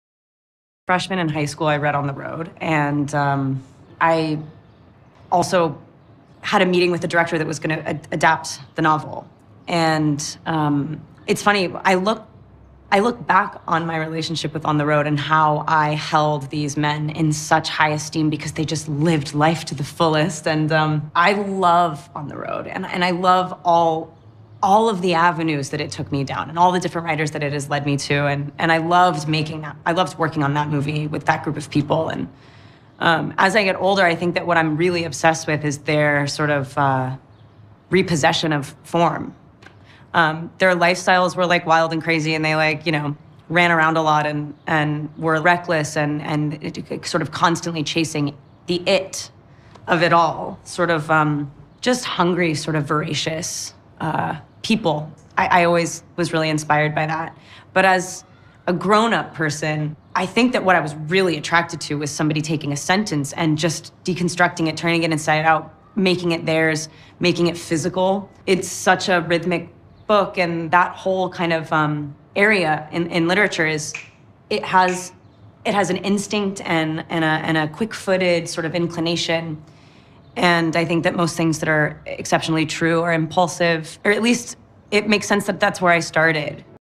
In the Library with KristenStewart | CHANEL р.2
"I love 'On the Road', and I love all of the avenues that it took me down and all the different writers that it has led me to."